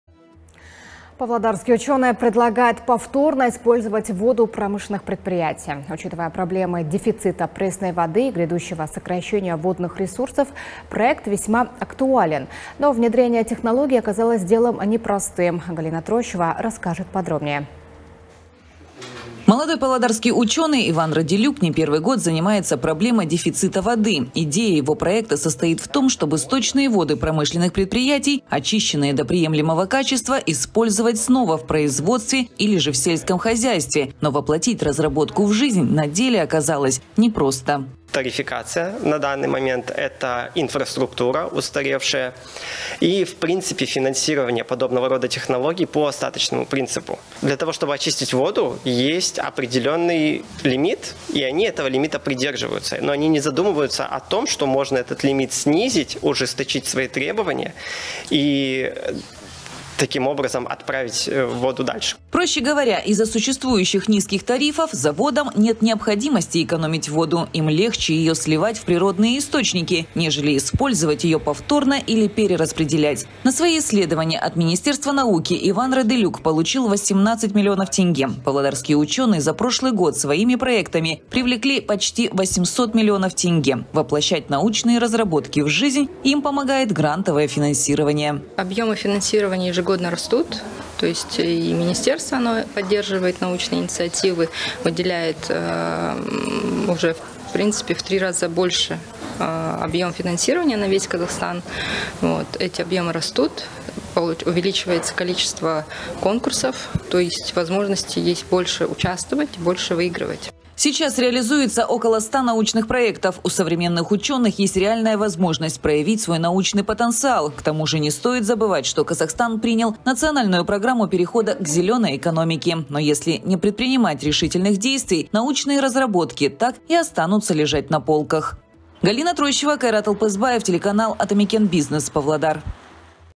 Павлодарский ученый предлагает повторно использовать воду предприятий
Павлодарский ученый предлагает повторно использовать воду промышленных предприятий. Учитывая проблемы дефицита пресной воды и грядущего сокращения водных ресурсов, проект весьма актуален. Но внедрение технологии оказалось делом непростым.